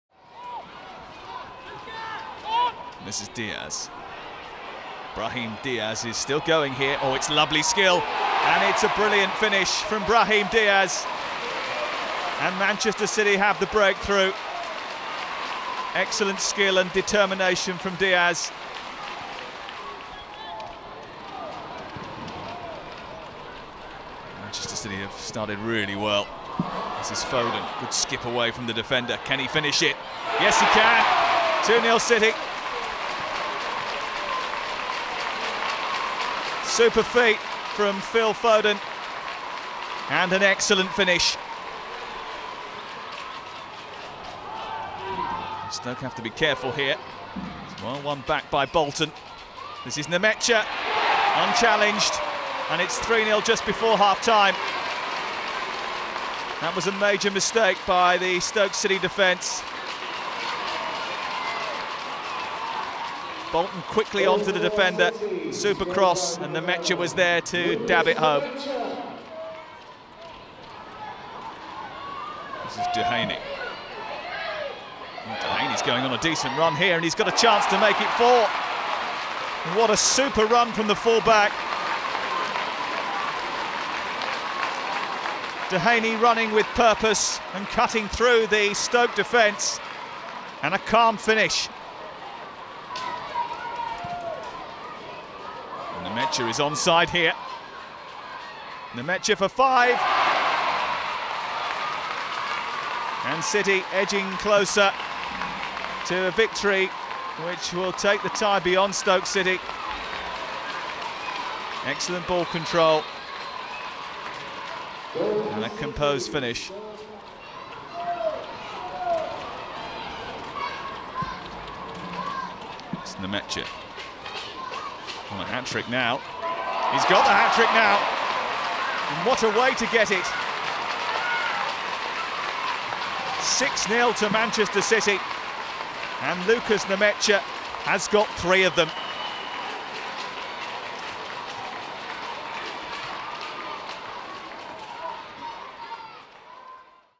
Man City 6-0 Stoke - 2016/17 FA Youth Cup semi-final First Leg | Official Highlights
Man City stormed to a 6-0 first leg win over Stoke City in their FA Youth Cup semi-final thanks to goals from Brahim DIaz, Phil Foden, Demeaco Duhaney and a hat-trick from Lukas Nmecha.